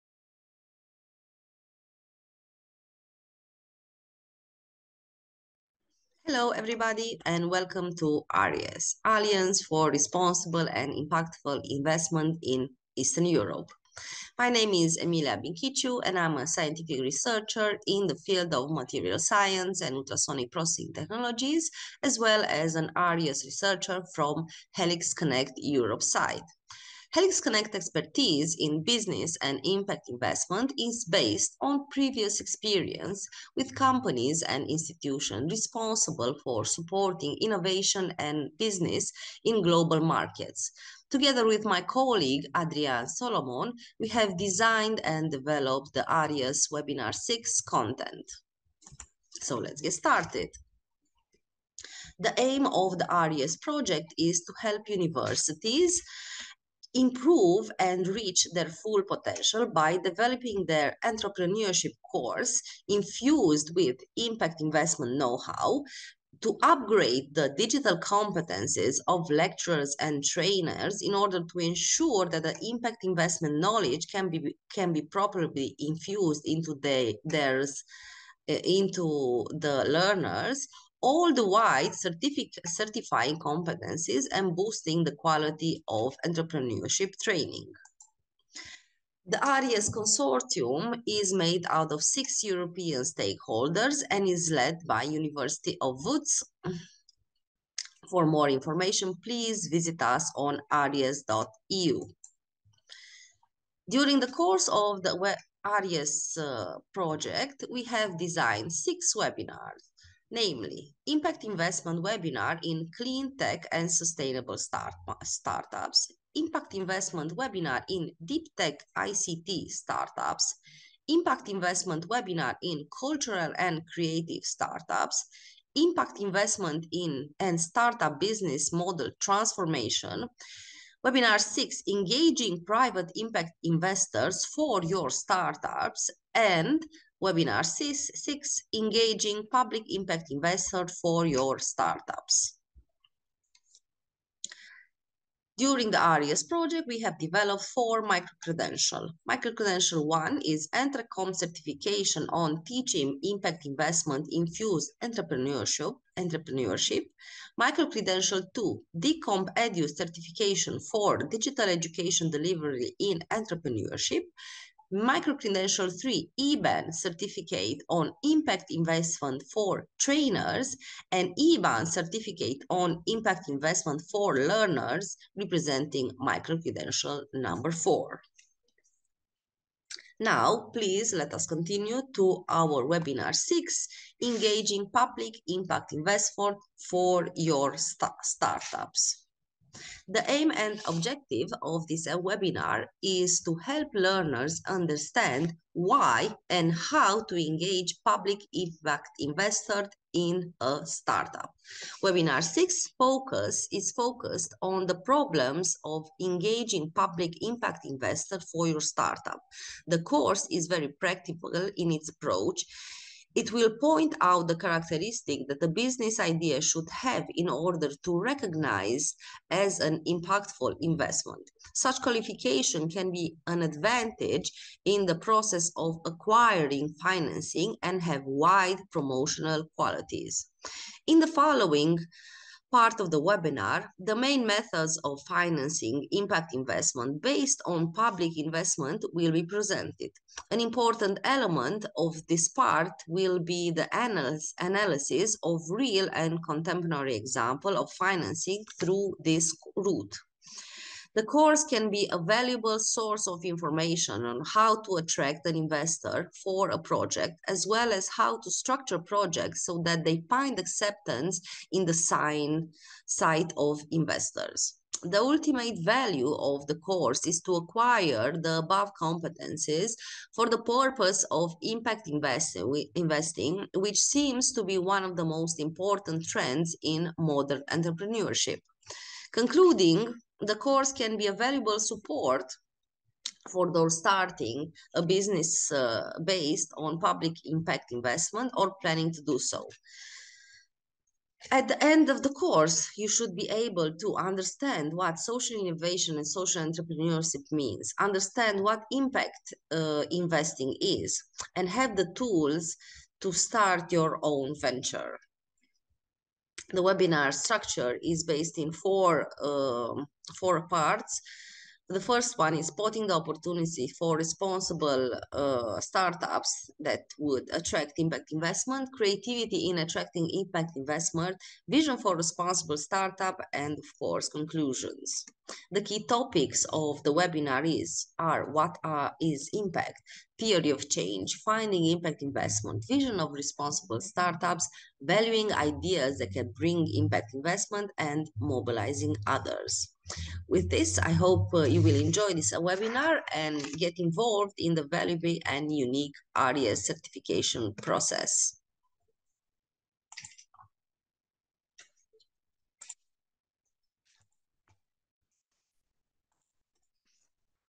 ARIEES WEBINAR 6 INTRO Engaging Public Impact Investors for Your Startup
Discover how to access public impact investment opportunities to fuel your startup's growth and impact goals. Our expert speakers will share their insights and experience on how to access public impact investment programs and showcase successful examples of startups that have secured public impact investments to support their growth and impact goals. Through engaging discussions and case studies, we will explore the strategies and best practices for engaging public impact investors, including how to identify public impact investment programs, how to apply for funding, and how to measure and report on impact.
Join us to discover how to access public impact investment programs and unlock the full potential of your startup.